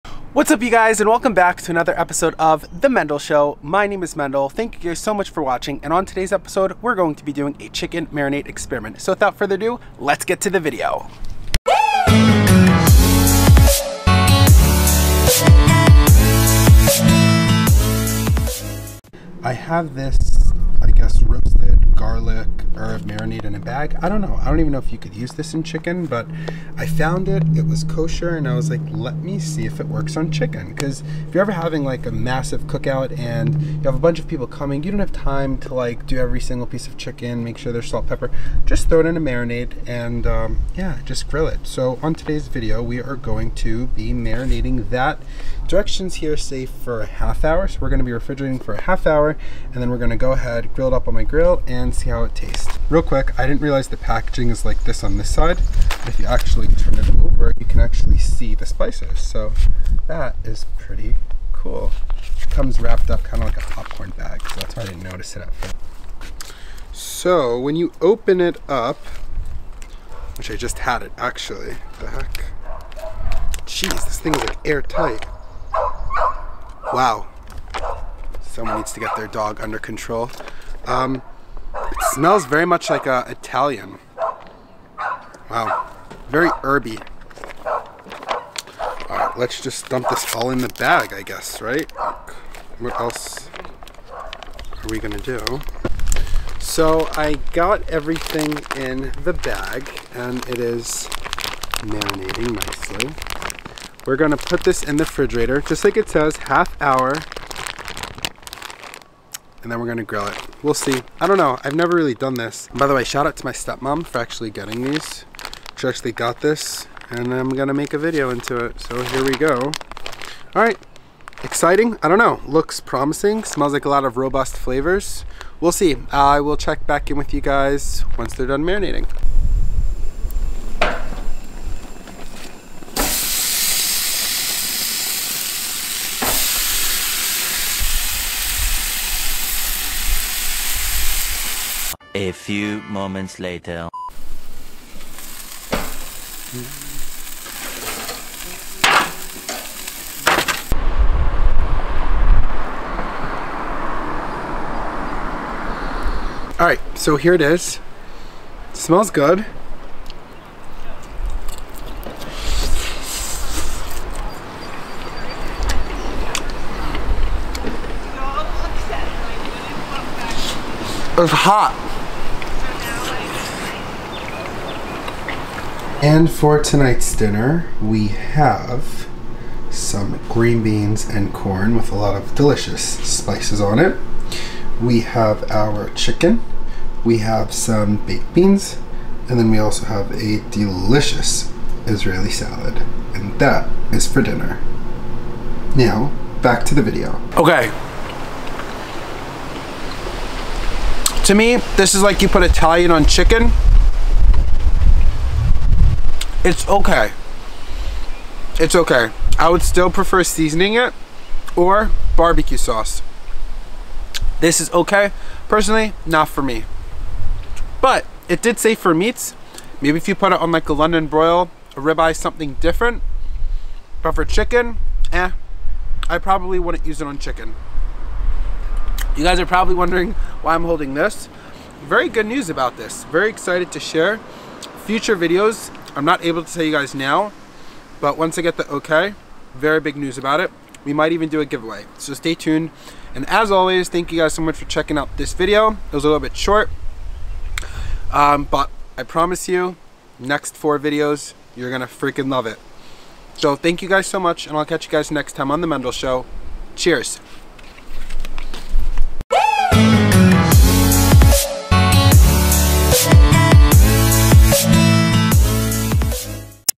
Grilled Chicken Marinade Experiment | 4K
What's up you guys and welcome back to another Episode of The Mendel Show! This today's episode is Roasted Garlic and Herb Marinade Experiment!

Thanks for being a part of the family!